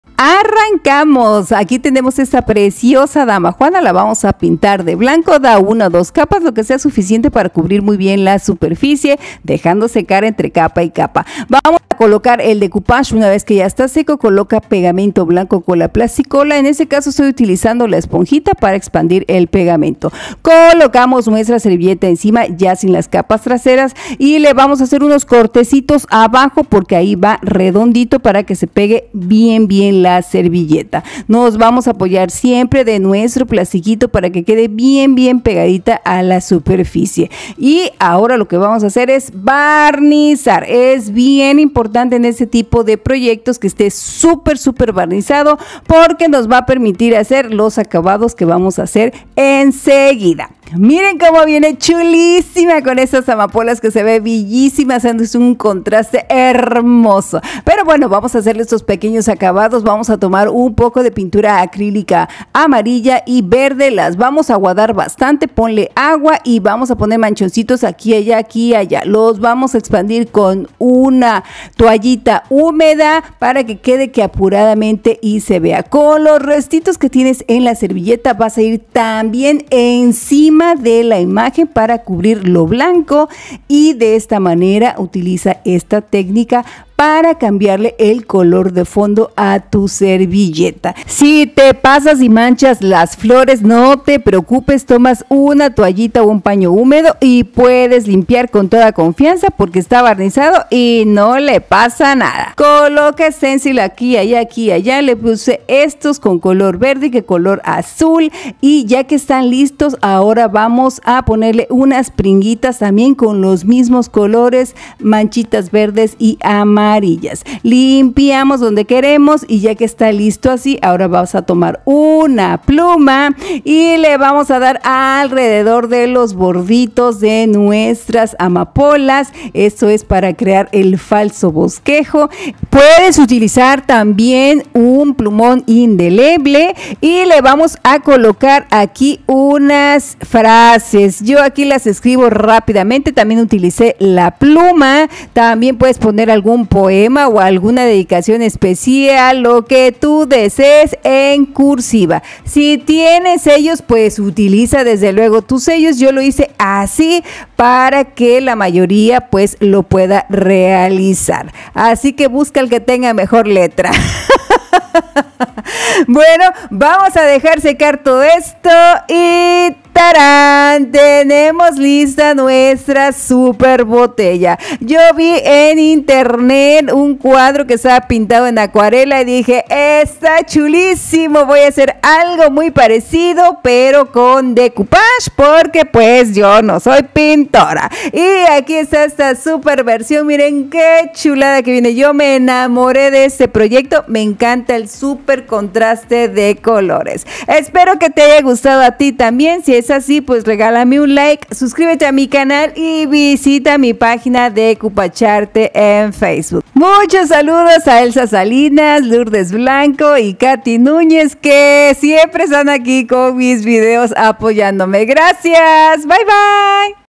BOTELLA DAMAJUANA DECORADA estilo ACUARELA
Decora damajuana y botellas con esta preciosa técnica estilo acuarela